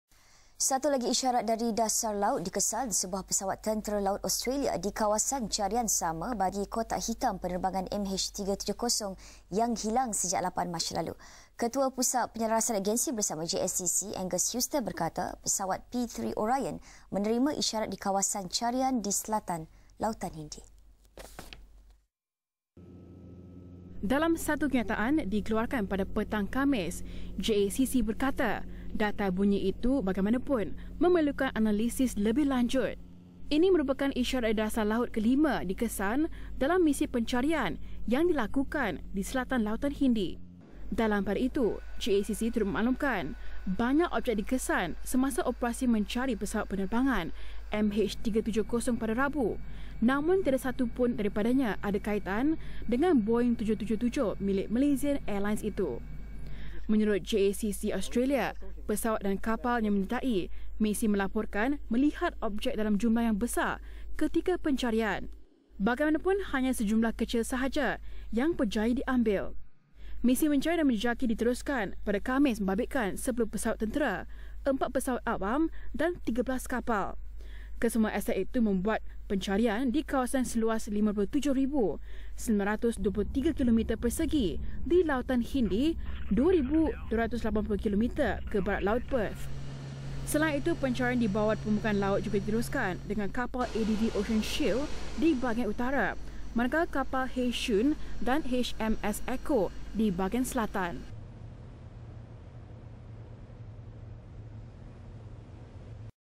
Lagi isyarat dikesan di dasar laut
Satu lagi isyarat dari dasar laut dikesan sebuah pesawat Tentera Laut Australia di kawasan carian sama bagi kotak hitam penerbangan MH370 yang hilang sejak 8 Mac lalu. Ketua Pusat Penyelarasan Agensi Bersama (Jacc) Angus Houston Berkata Pesawat P-3 Orion Menerima Isyarat Di Kawasan Carian Di Selatan Lautan Hindi.